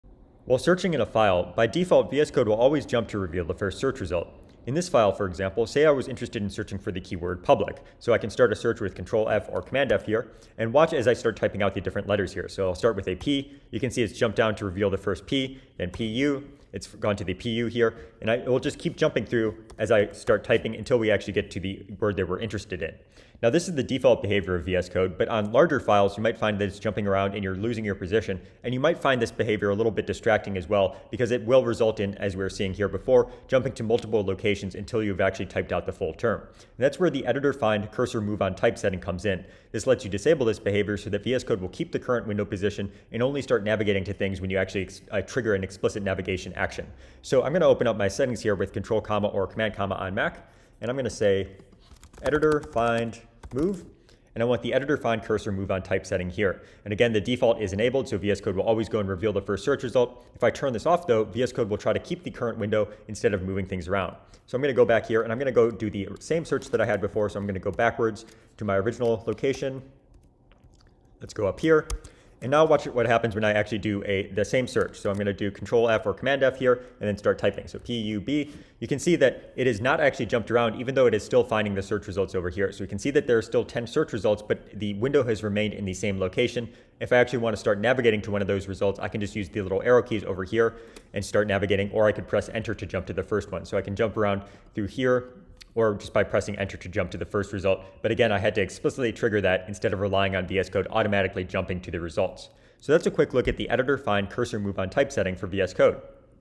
VS Code tips — The Editor: Find: Cursor move on type setting
Today's VS Code setting — Editor: Find: Cursor move on type

Disable VS Code automatically revealing search matches as you type in the search box.

Useful if you want to keep your place in a file while searching or find all the jumping around while searching distracting.